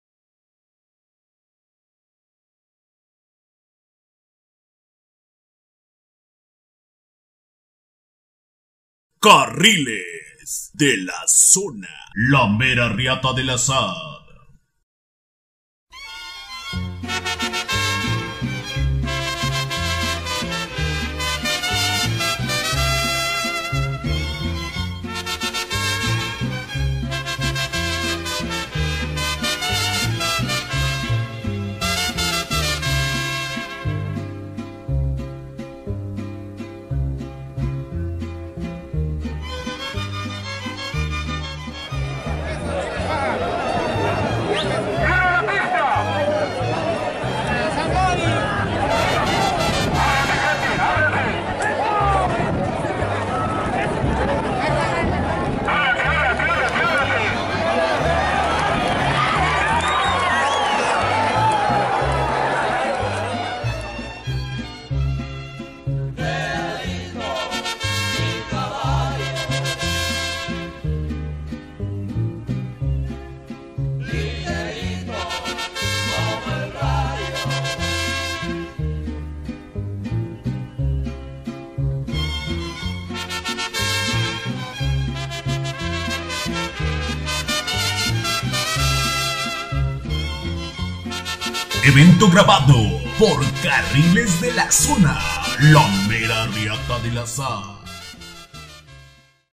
EL SARGENTO VS EL VIANKO
Ganando en 200 varas Tp
EL SARGENTO a el vianko
LOS TIGRES
25/4/17